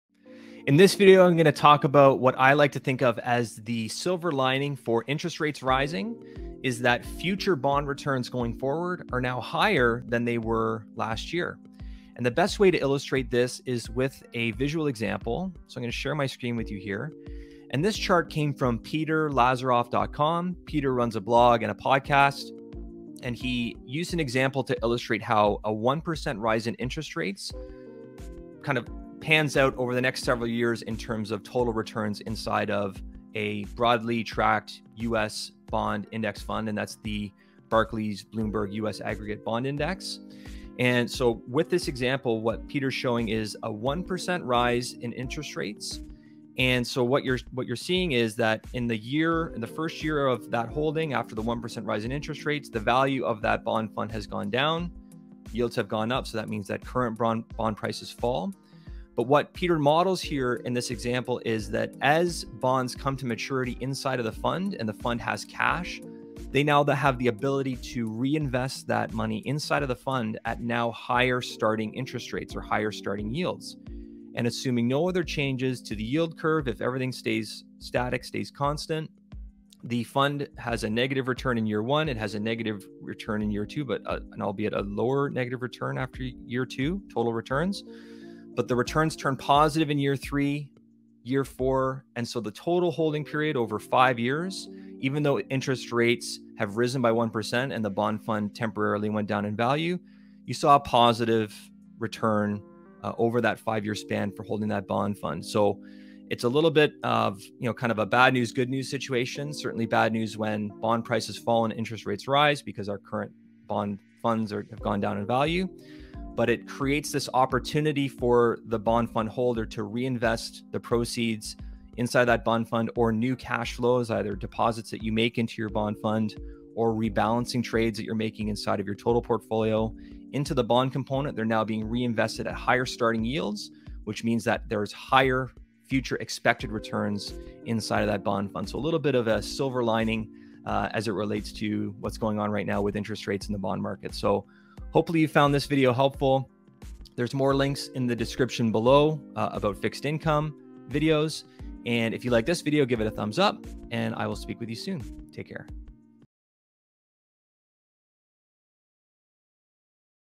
Silver Lining for Rising Rates
The one cool thing that happens when rates rise is that maturing bond cashflows can be reinvested at now-higher starting yields.  I walk through a visual example of how this works.